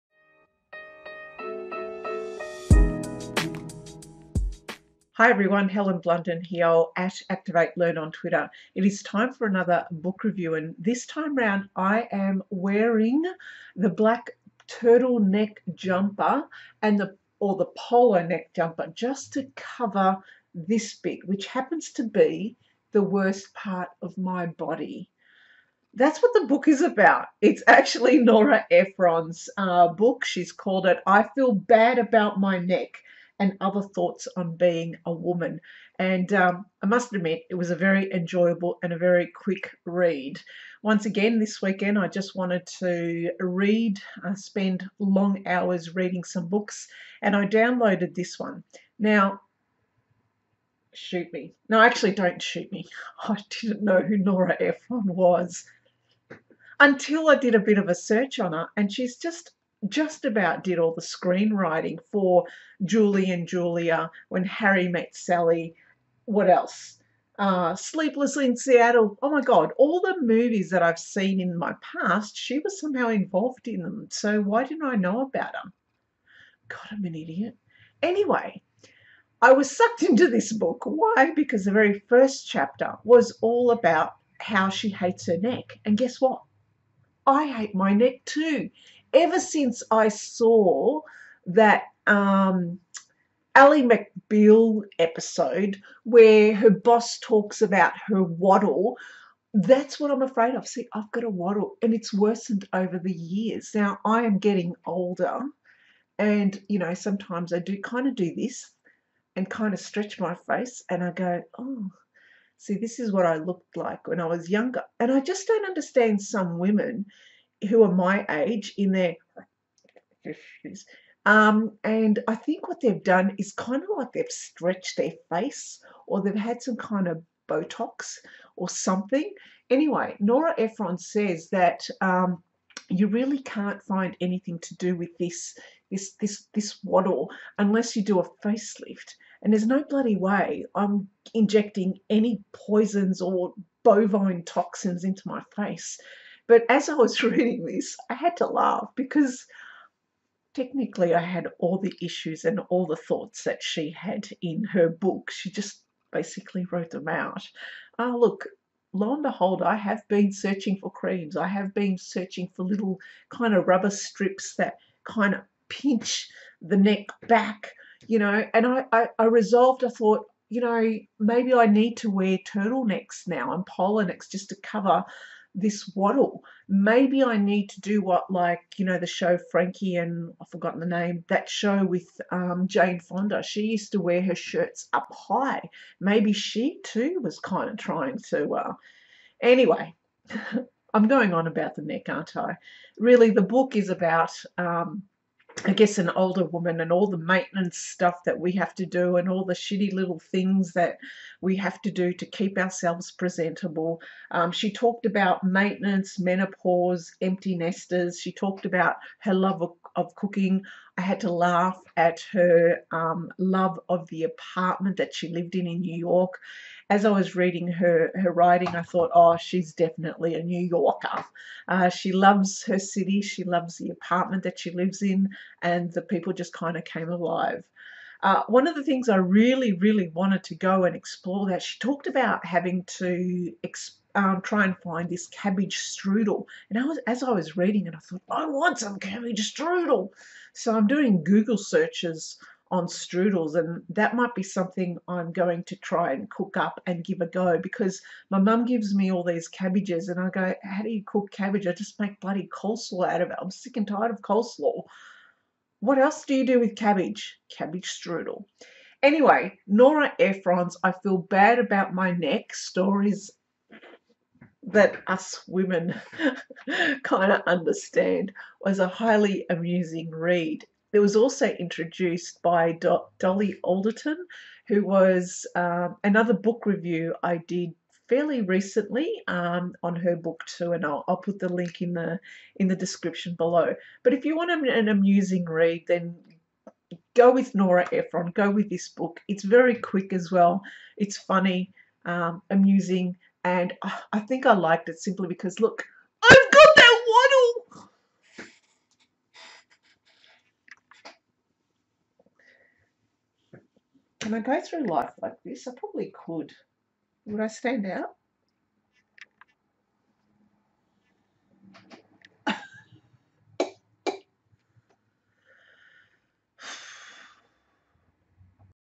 Book Review On I Feel Bad about My Neck By Nora Ephron
Hello everyone and welcome to another book review.

This one was a lot of fun for me and a quick read. I had no idea who Nora Ephron was but glad I bought and downloaded this hilarious read.  If you want a book that makes you forget for a few hours and immediately sucks you in then this is it.  Pretty lighthearted perspective to ageing.